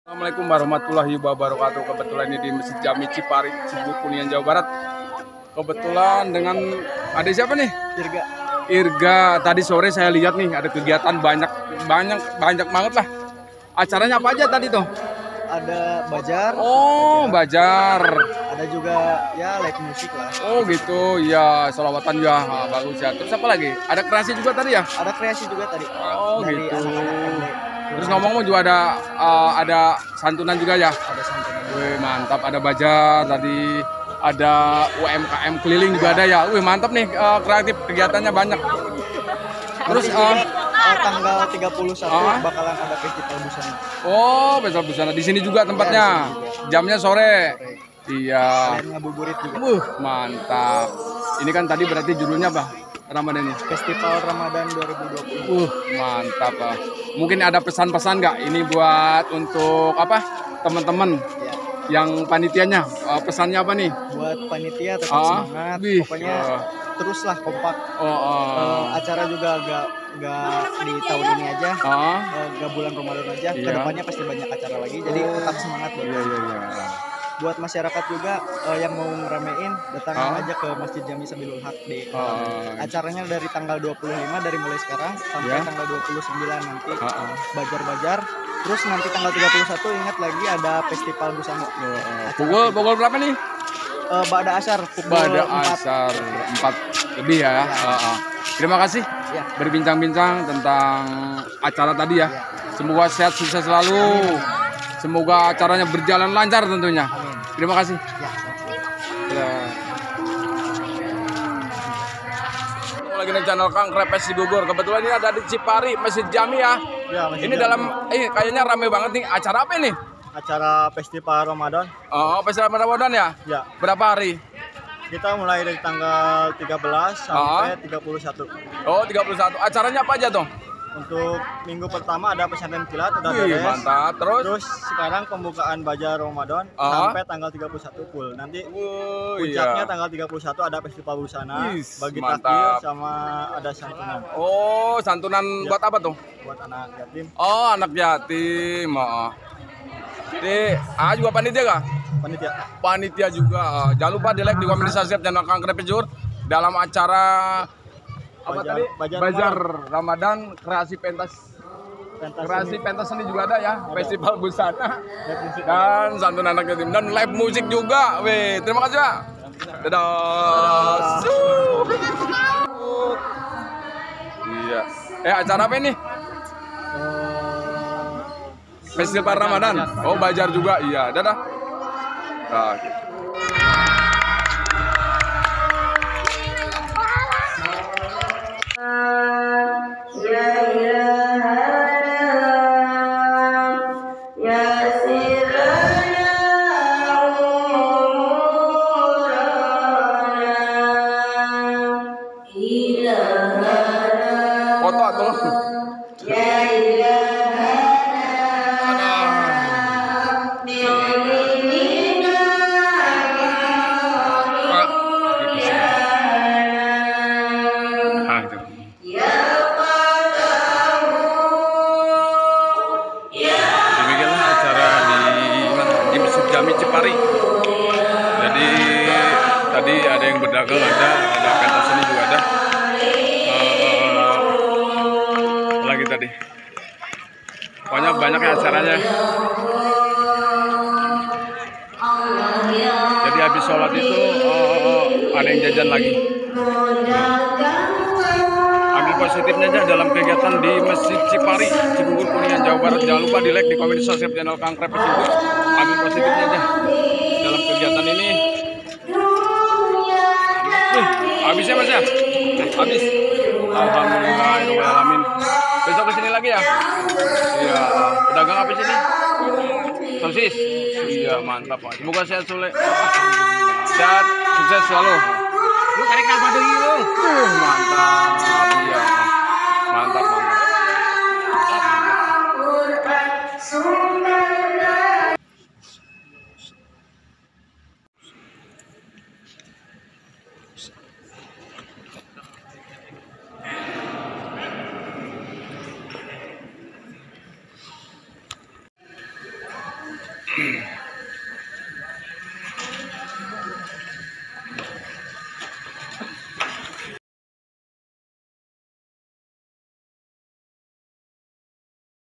Mesjid Jami Cipari festival ramadhan || Part 1
Acara di mesjid Jami Cipari di bukan Ramadan banyak kegiatan dan bazar